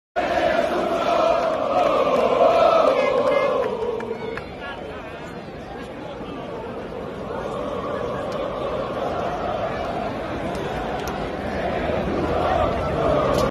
Invasione rossonera a CASAMILAN ! ♥️🖤
A casamilan si festeggia lo scudetto per il secondo giorno di fila